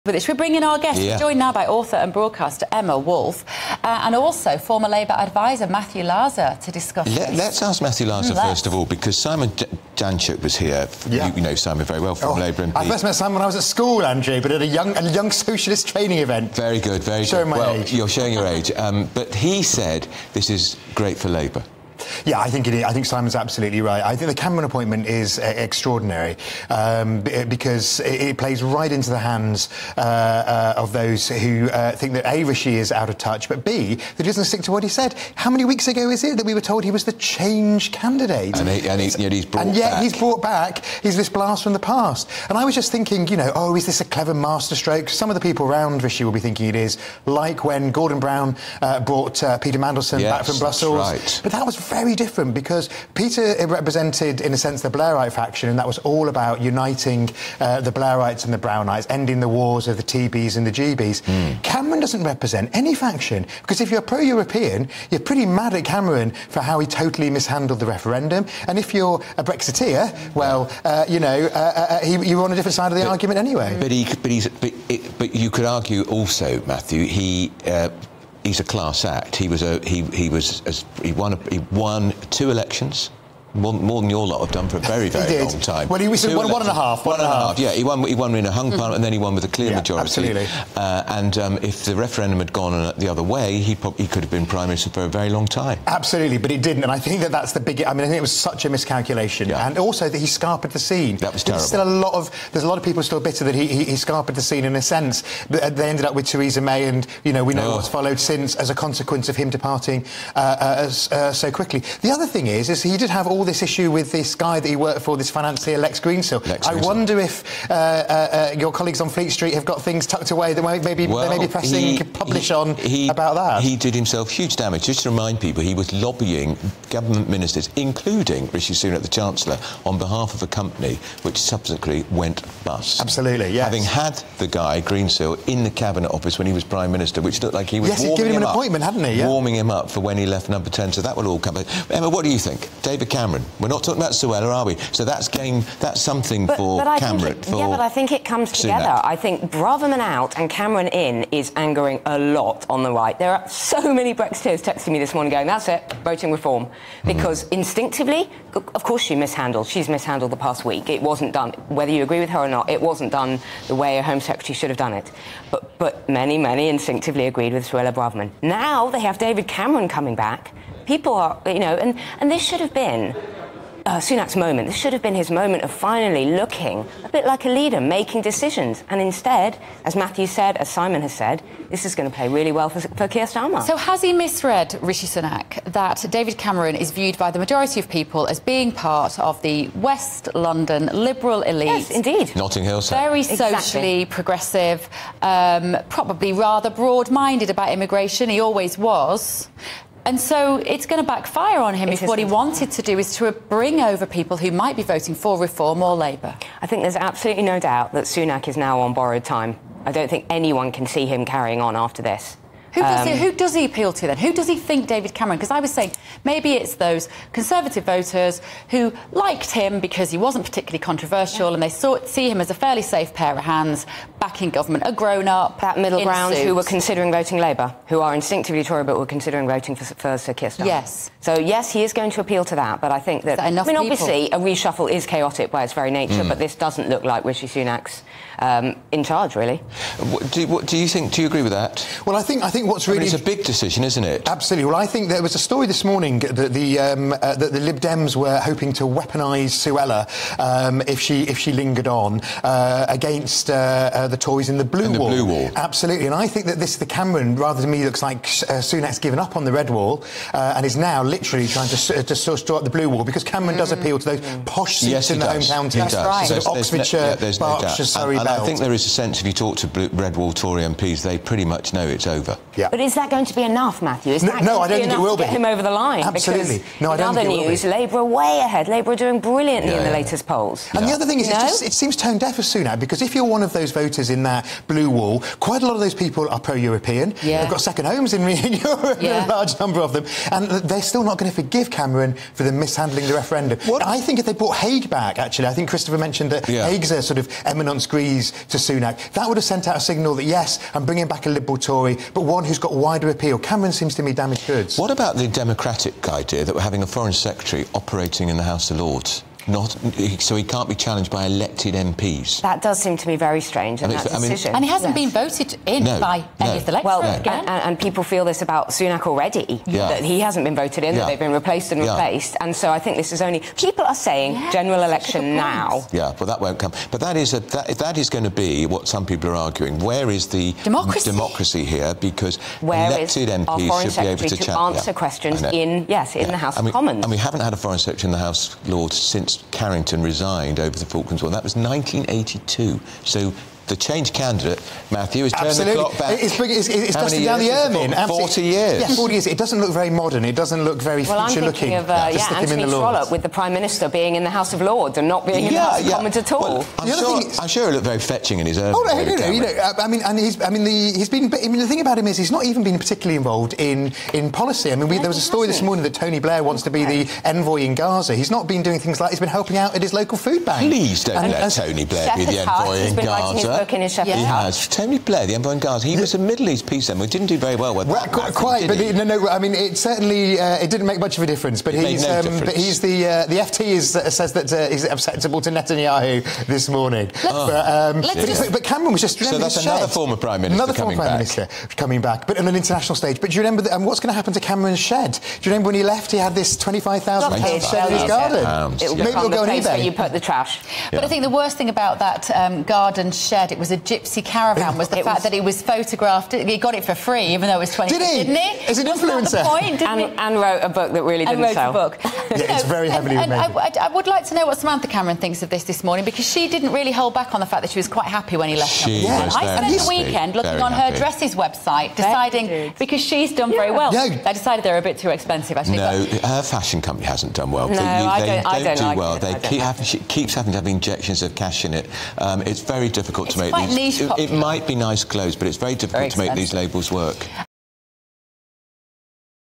'Sunak is on BORROWED TIME' after Suella sacking: 'He can't carry on after this!' | Emma Woolf
'Braverman out and Cameron in is angering a lot of people on the right! This should've been Sunak's moment, but now it's going to play well for Starmer.'

Author, Emma Woolf, says Rishi Sunak is 'on borrowed time.'